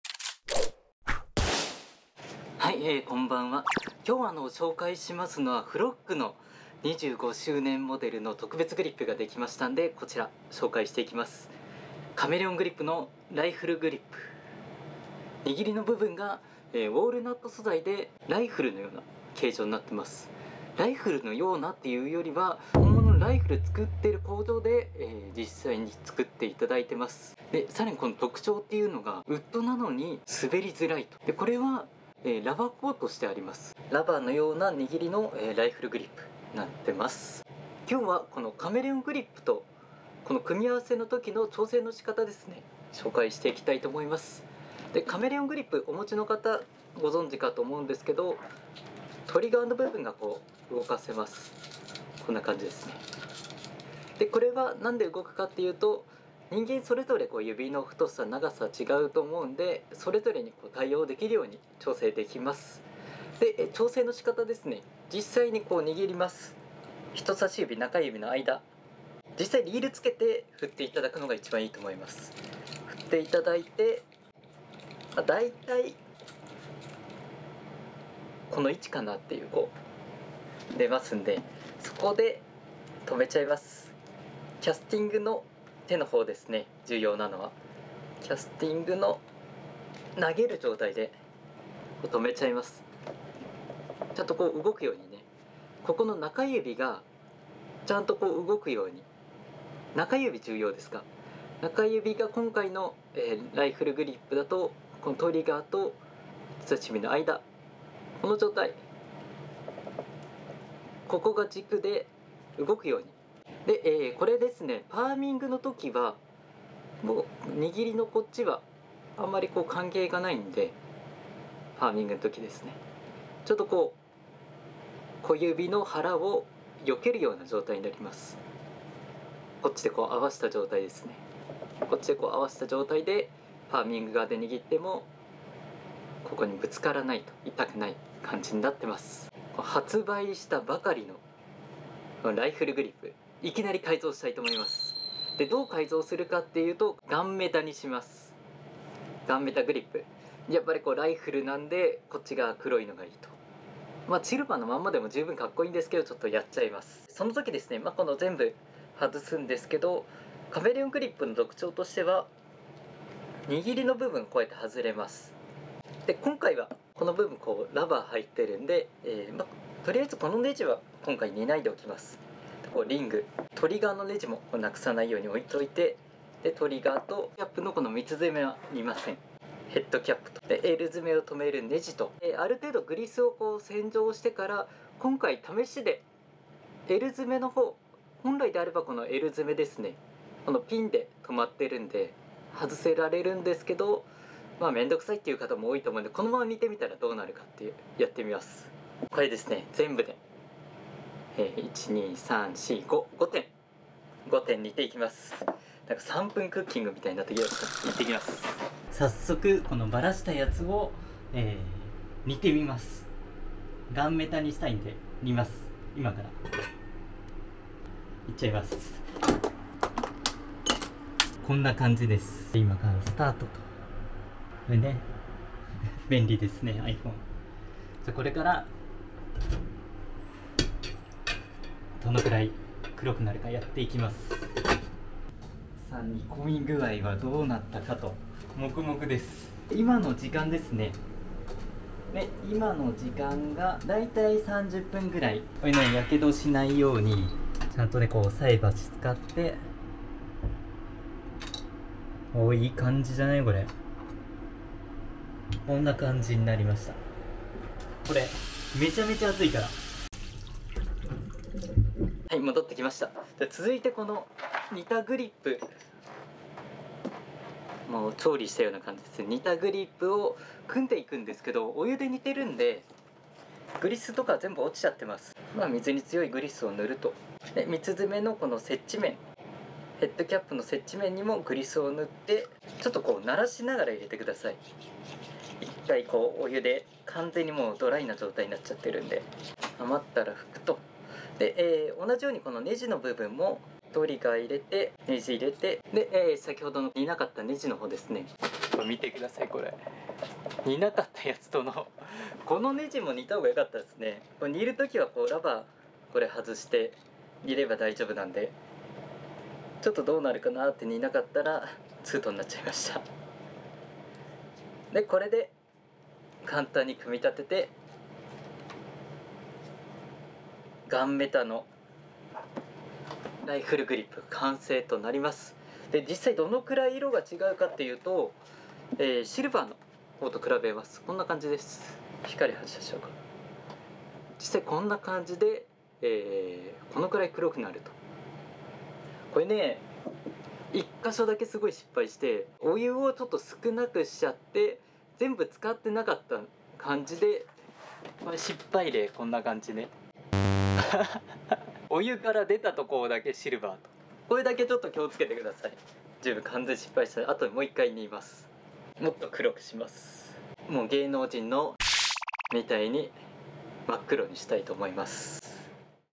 ライフル職人が作る釣具！FROGカメレオンライフルグリップ
発売したばかりのFROGライフルグリップをいきなり改造。 金属部分をガンメタにしてみました。 ライフル　釣具